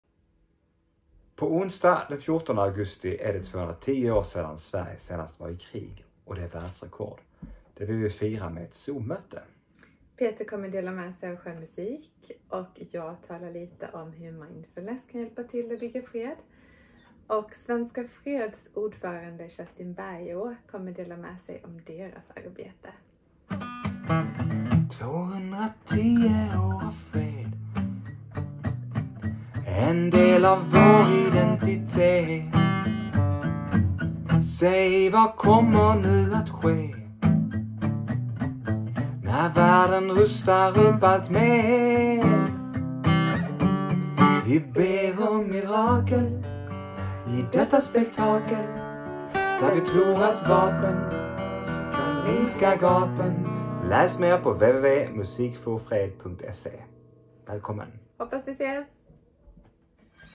Sånger och samtal om fred - 14 aug 2024
Den 14 aug 2024 är det 210 år sedan Sverige senast var i krig. Detta vill vi uppmärksamma genom ett online event bestående av musik och reflektioner kring fred som avslutas med samtal i smågrupper.
Medverkande:
Kerstin Bergeå (Svenska Fred)
Peter Elmberg (musiker, projektledare)
Anna Rasmussen (mindfulness inspiratör)
Arrangör.
Musik för fred i samarbete med Svenska Freds och Skiljedomsföreningen